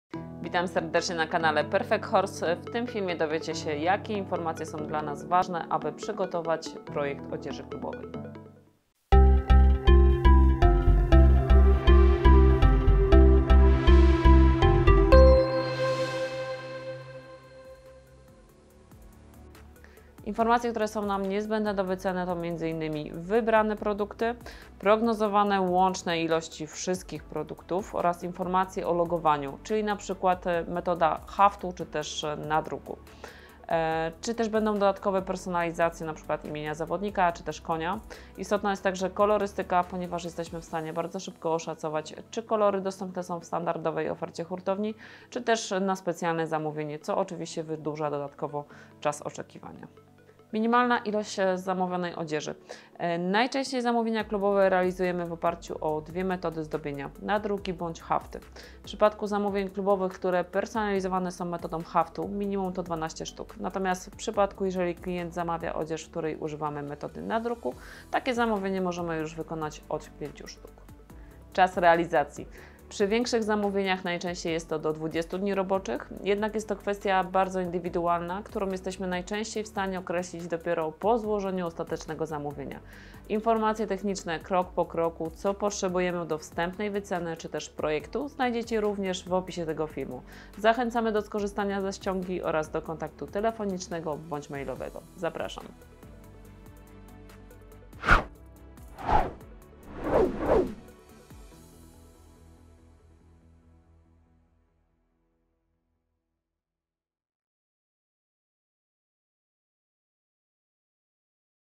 Jak złożyć zamówienie na odzież klubową/ firmową- co jest potrzebne do wyceny?
Istotne informację jakie potrzebujemy do wyceny:
1. Wybrane produkty np: czapka, kurtka, koszulka polo, bluza dresowa, czaprak
2. prognozowane łączne ilości wszystkich produktów
3. informację o logowaniu (jaka metoda, hafty/ nadruki/ inne)
4. dodatkowa personalizacja (np imiona zawodników/koni)
5. kolorystyka produktów

minimalna ilość dla zamówien personalizowanch metodą haftu:
od 12 szt
minimalna ilość dla zamówien personalizowanch metodą nadruku:
od 5 szt
czas realizacji do 20 dni roboczych przy zamówieniach powyżej 50 szt.
zamówienia oraz zapytania ofertowe prosimy kierować na
Zapraszamy :)